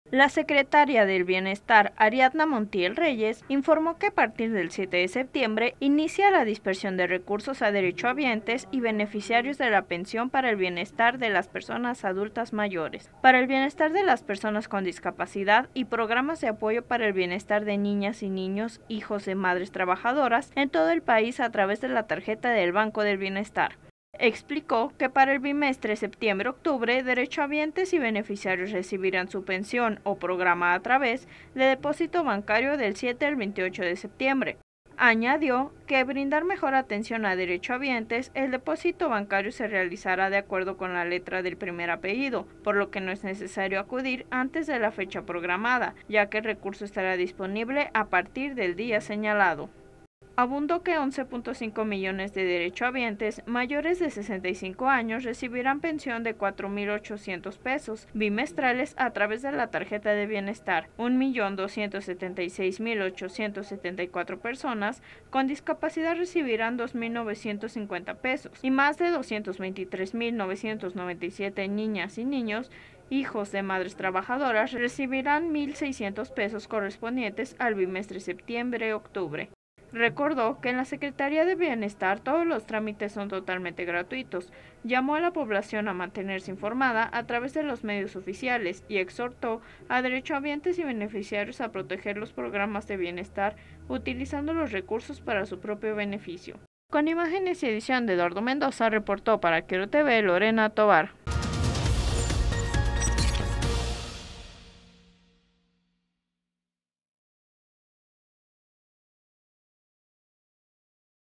Pago de pensiones
Inicia Secretaría de Bienestar pago de pensiones y programas del bimestre septiembre-octubre 2023.